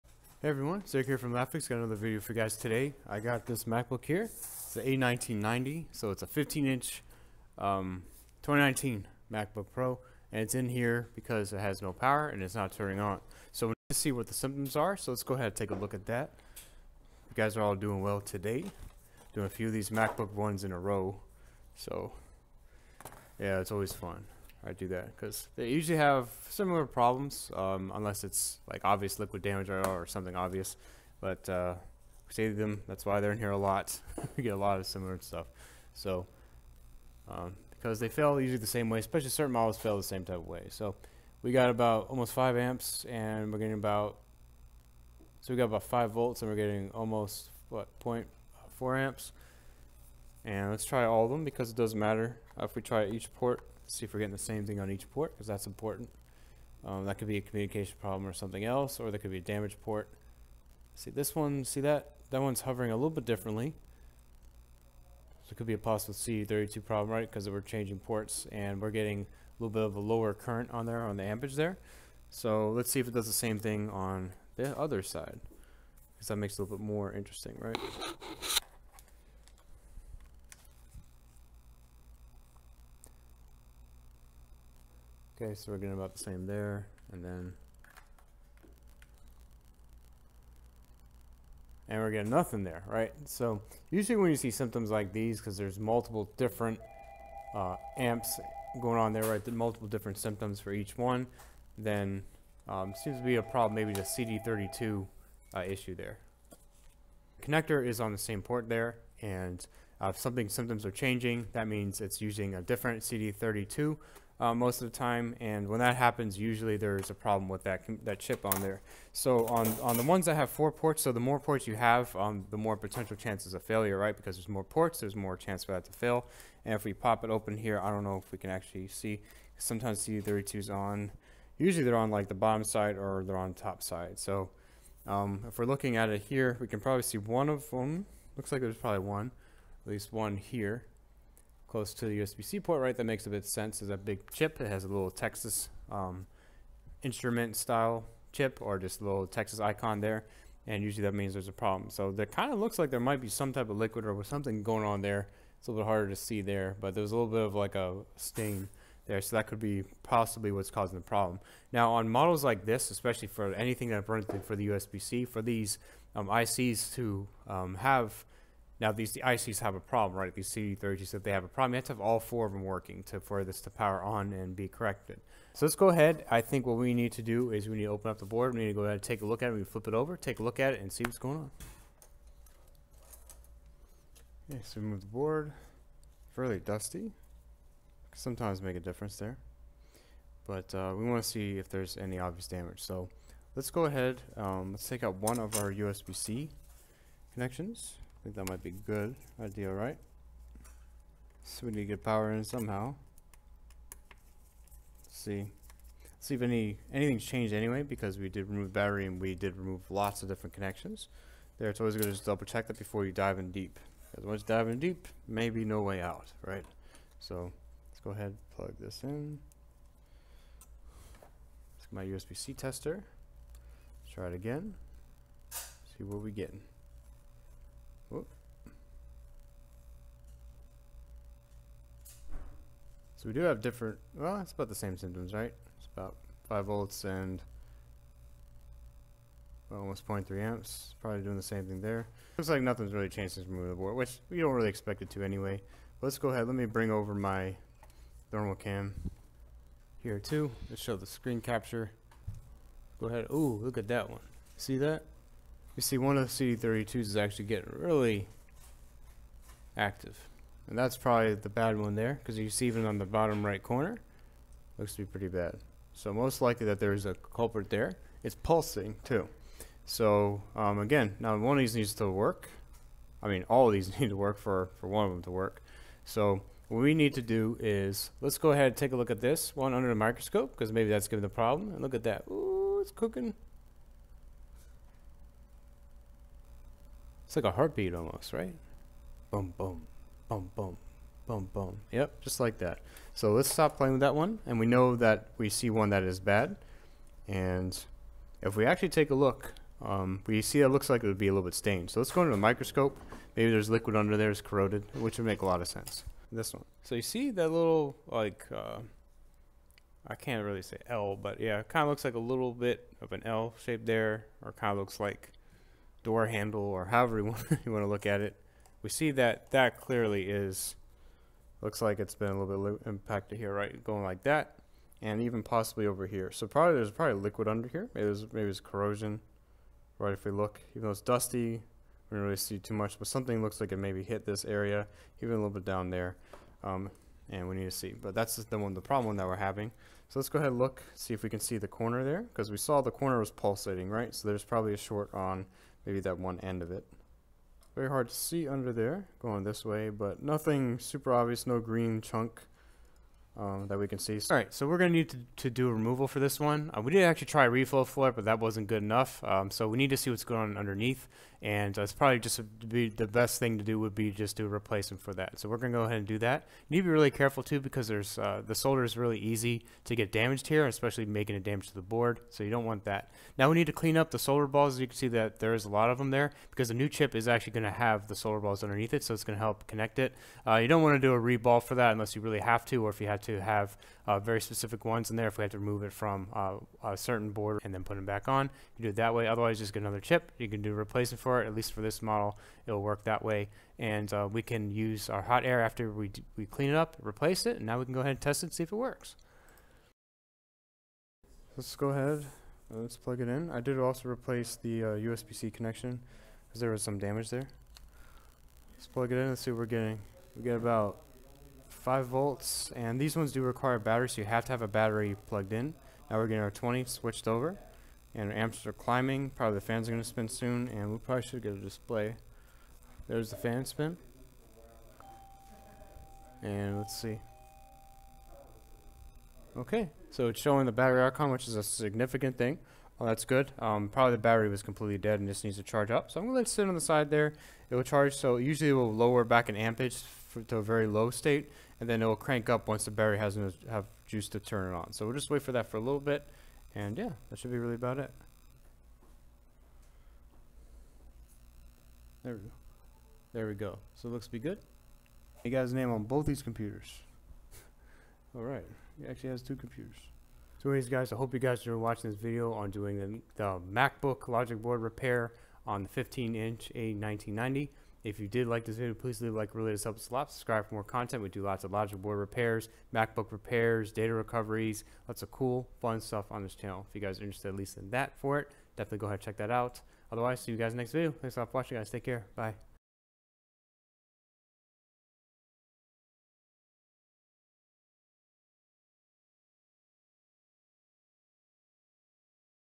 How to Fix a 15 inch MacBook Pro Not Charging
Hey everyone, today we are looking at How to Fix a 15 inch MacBook Pro Not Charging